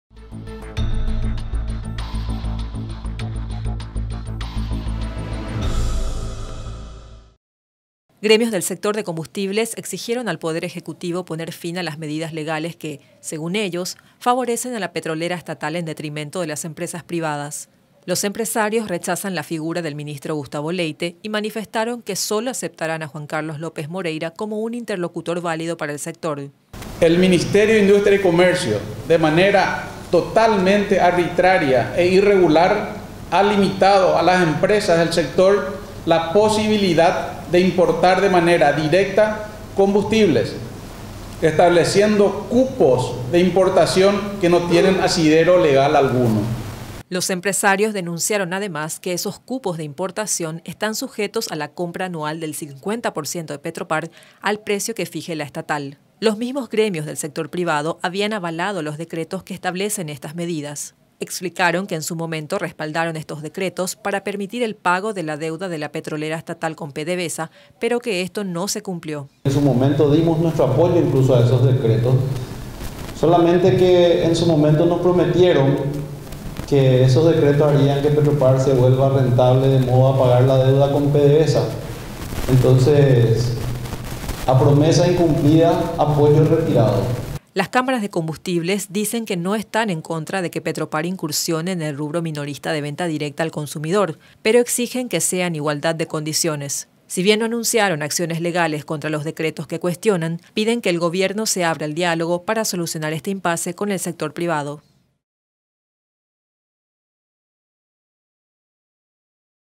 Representantes de gremios privados en contra del ministro del Industria
Empresarios del rubro de combustibles manifestaron su rechazo a la gestión del ministro de Industria y Comercio, Gustavo Leite, a quien acusan de operar para favorecer sólo a Petropar. Piden que el Ejecutivo derogue una serie de decretos sobre la regulación de importación y venta.